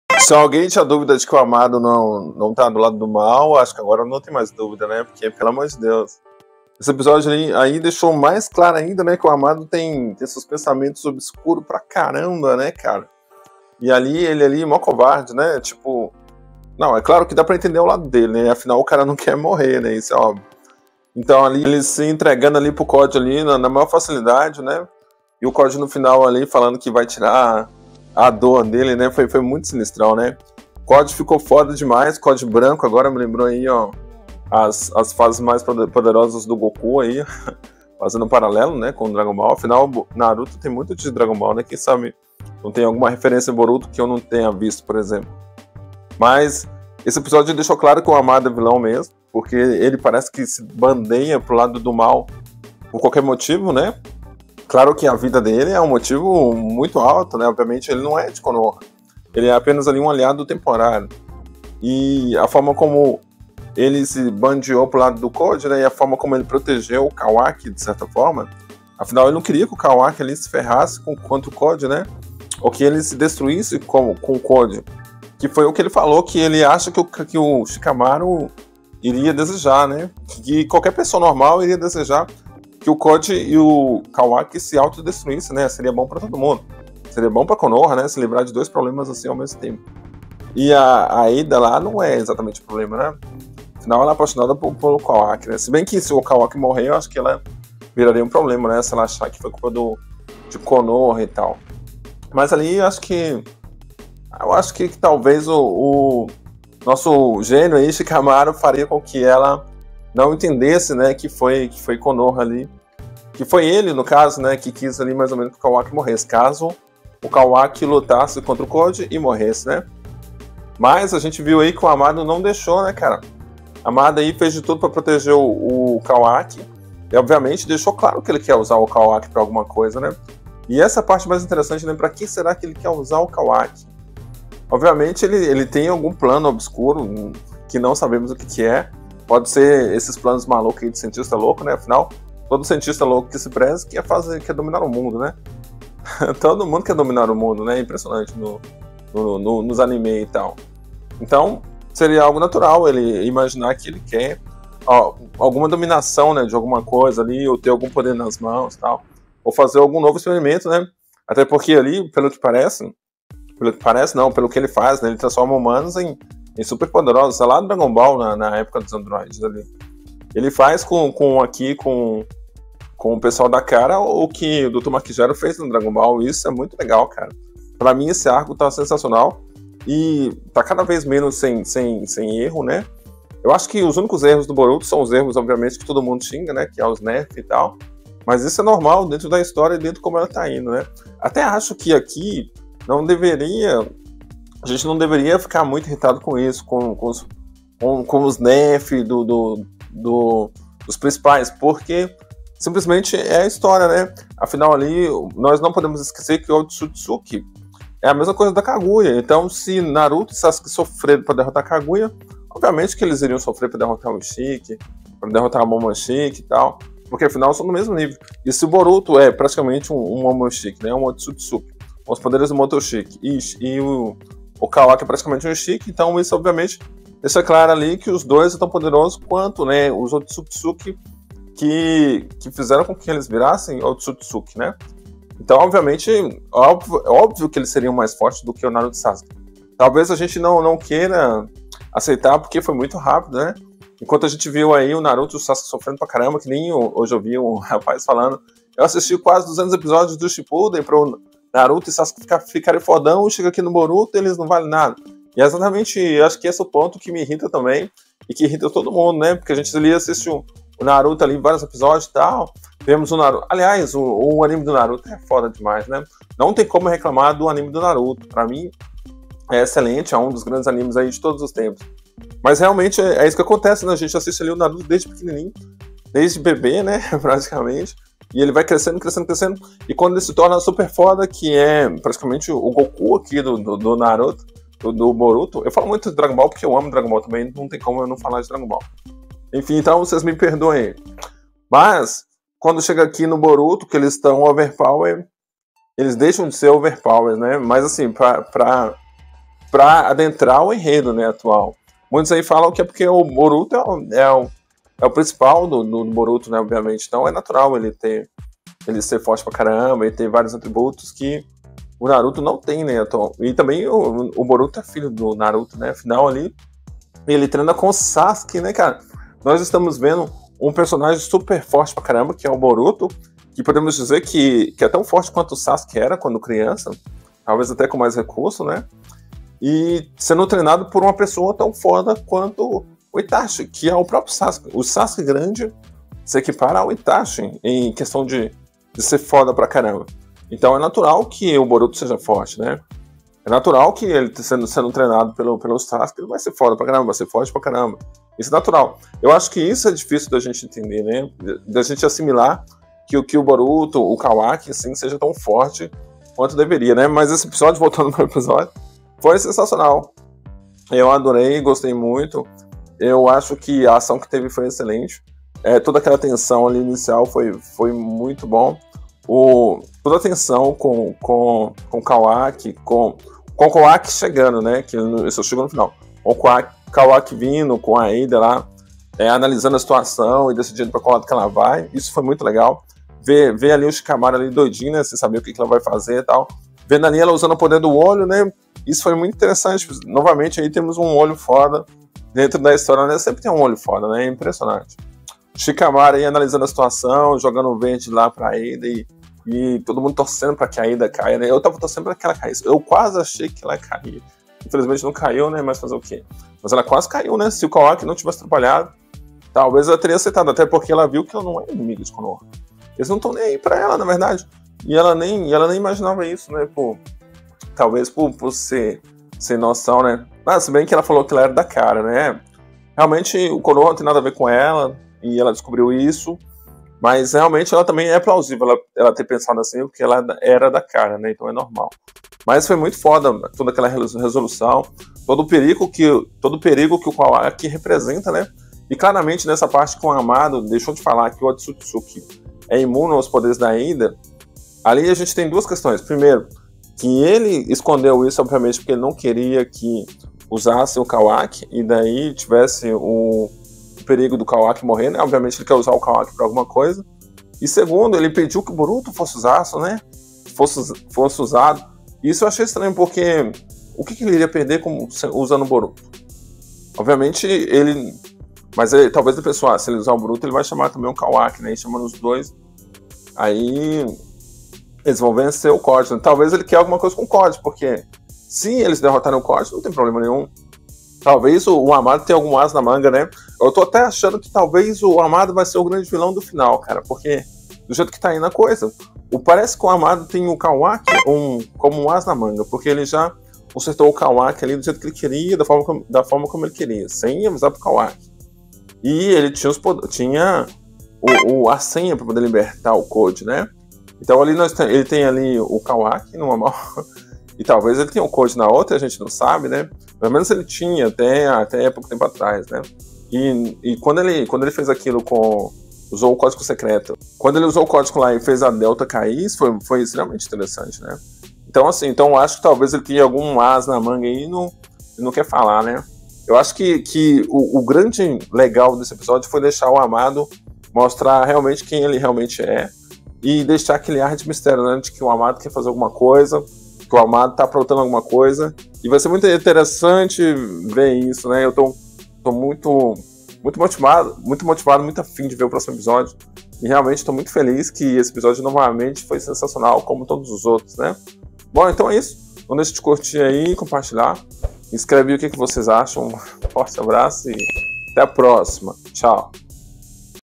BORUTO 70 - AMADO VILÃO ? CODE OVERPOWER!
Boruto 70 ! O mangá está aí arrebentando !
Como sempre !